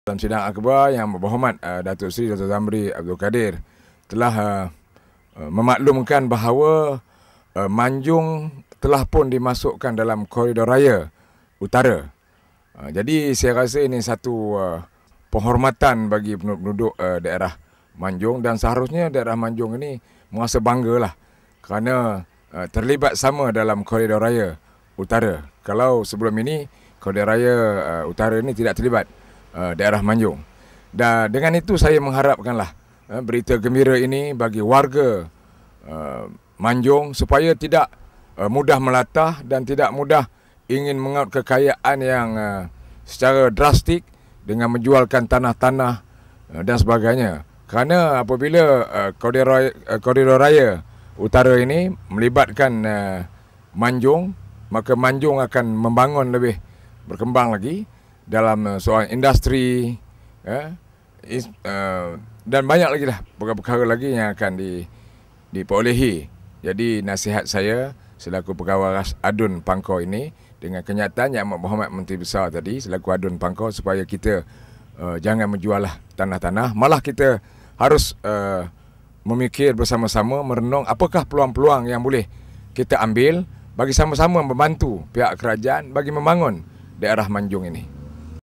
Rakyat Manjung Sambut Baik Pembangunan Koridor Utara di Daerah Itu:- Azri Jaafar
30/01/14-- Rakyat Manjung Sambut Baik Pembangunan Koridor Utara di Daerah Itu:- Azri Jaafar

Pengumuman kemasukan daerah Manjung sebagai salah satu kawasan dalam pembangunan Koridor Utara ternyata diterima baik oleh penduduk kawasan itu.

Naib Ketua UMNO Bahagian Lumut, Azri Jaafar berkata, berita baik berkenaan diterima dengan rasa bangga dan berharap pembangunan di Manjung dapat dimantapkan pada masa akan datang.

"Menteri Besar telah memaklumkan bahawa Manjung telahpun dimasukkan dalam Koridor Utara.

"Jadi saya rasa ini satu penghormatan bagi penduduk daerah Manjung.

"Kami berasa bangga kerana terlibat sama dalam perancangan pembangunan Koridor Utara," katanya.

Menurut beliau, sebelum ini daerah Manjung tidak terlibat dengan pembangunan Koridor Utara.

Justeru, dengan wujudnya pembangunan Koridor Utara di daerah berkenaan mengharapkan berita gembira itu tidak diambil kesempatan oleh segelintir pihak.

"Saya harap bagi warga Manjung supaya tidak mudah melatah dan tidak mudah ingin mengaut kekayaan secara drastik dengan menjual tanah-tanah dan sebagainya.

"Ini kerana apabila Koridor Utara terlibat di Manjung maka daerah ini akan lebih berkembang lagi.

"Perkembangan dan kemajuan itu boleh meliputi pelbagai termasuk dalam soal industri dan banyak lagi yang akan diperolehi.

"Jadi nasihat saya supaya kita jangan menjual tanah malah kita harus merenung bersama apakah peluang-peluang yang boleh diperoleh kita ambil untuk membantu kerajaan," katanya.